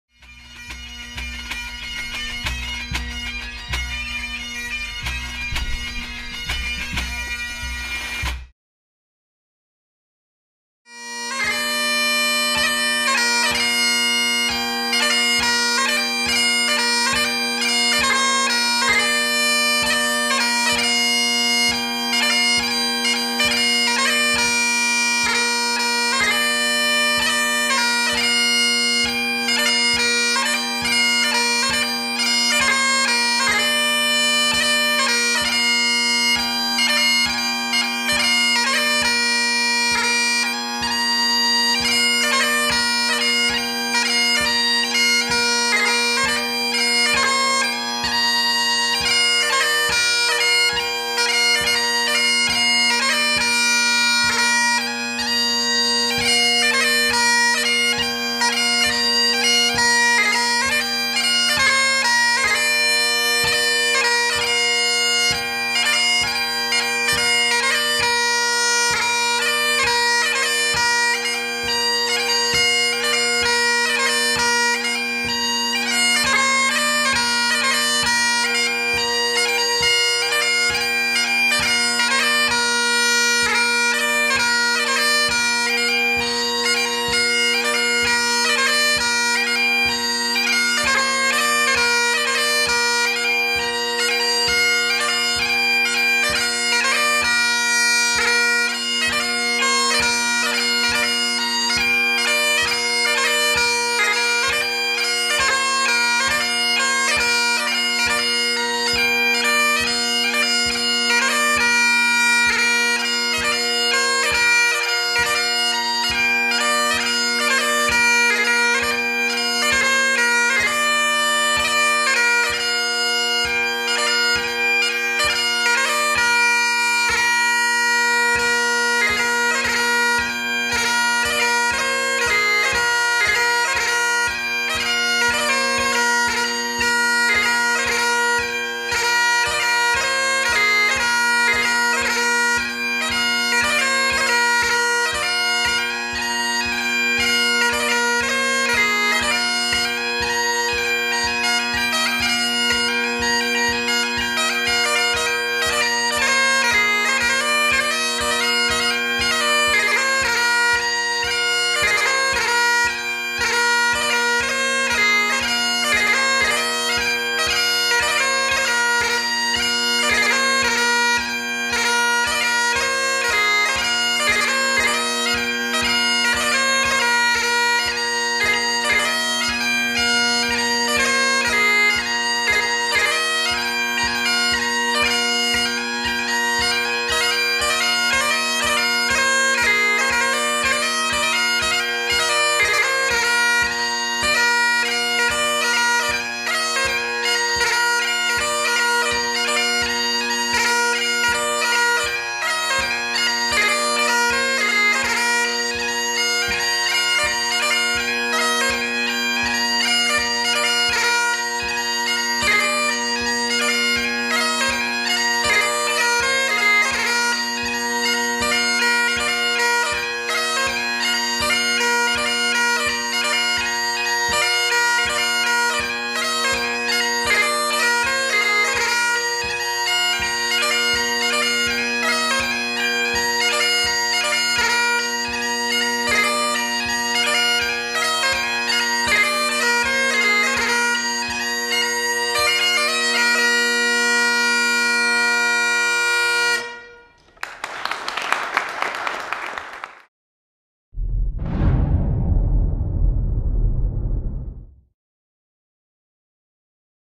Austin Diepenhorst - 2015 Sherriff Memorial
Austin Diepenhorst of San Diego competing in the March, Strathspey & Reel event at the 2015 George Sherriff Memorial Amateur Invitational Solo Piping Competition, November 21, 2015, at St. Paul's Church, Hamilton Ontario. Diepenhorst finished first in the event and first overall.